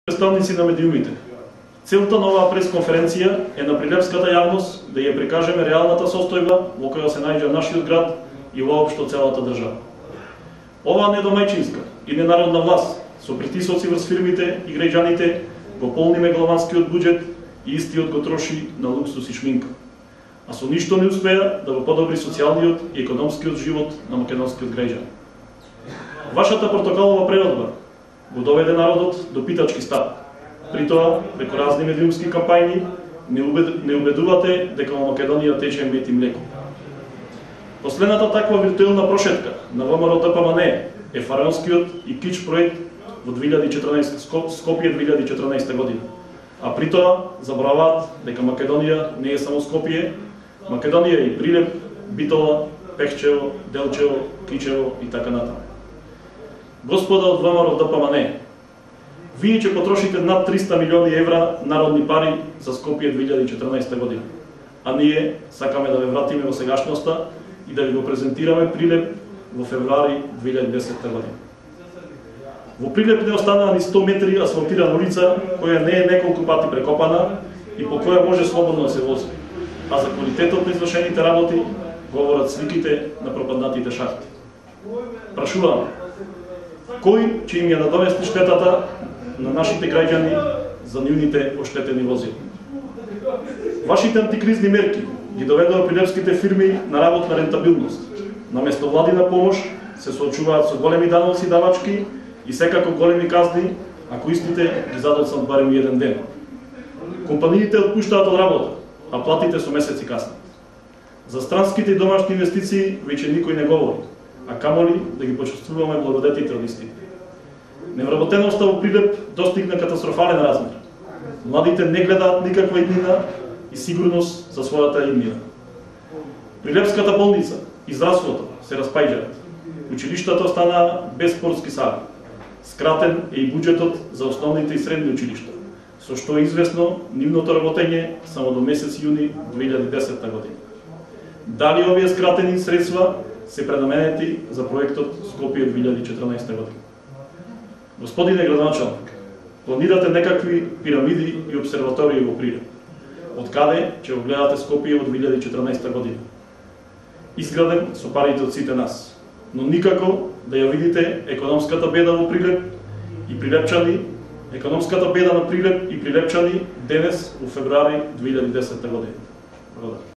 СДСМ- ПРИЛЕП Пресконференција 23.02.2010
Пресконференција одржана од секретарот на О.О. на СДСМ од Прилеп Звонко Јованоски за состојбата во која се наоѓаат Прилепските улици.